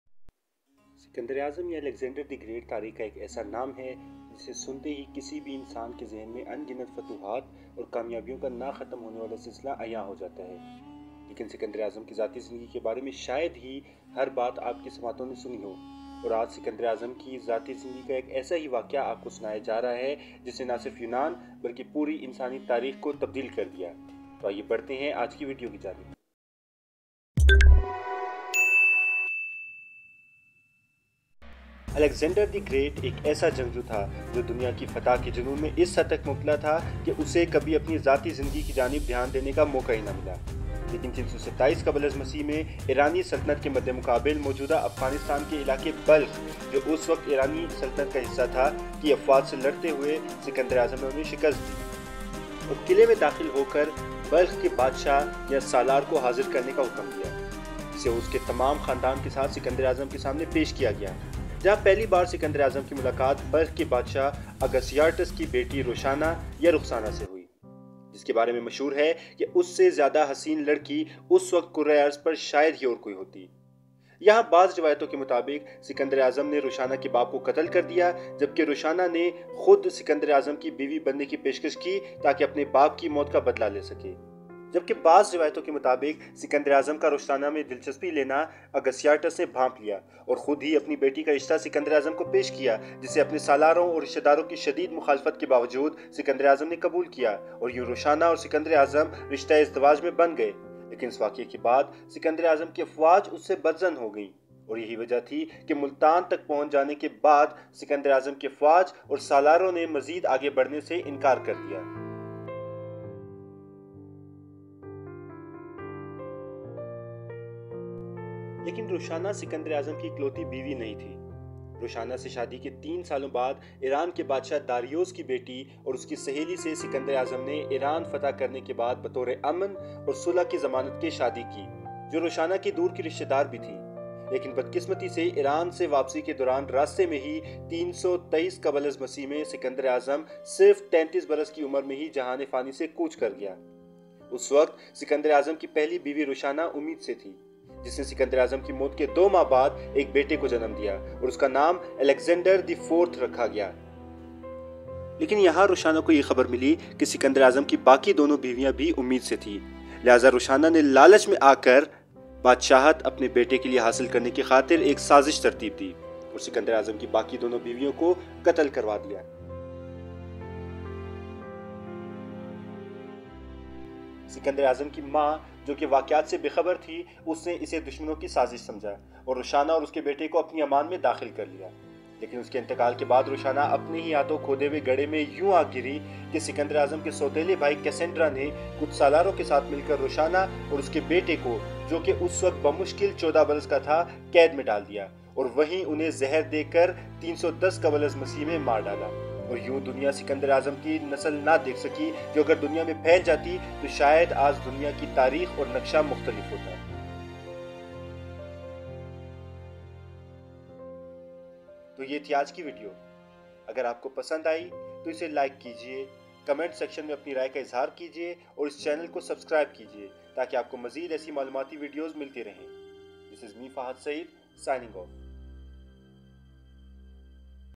Tragic Story of Alexander & Roxana || Did Alexander Have Any Child? || In Urdu/Hindi
Who Was Roxana? What was her relation with Alexander The Great? Did Alexander Have any child? If Yes, Where did it go in history? Find all the answers in the video along with many interesting and astonishing facts about Alexander and his personal life.
A story from Personal life of Alexander The Great who ruled half the Earth.

Researched & Narrated By: FAHAD SAEED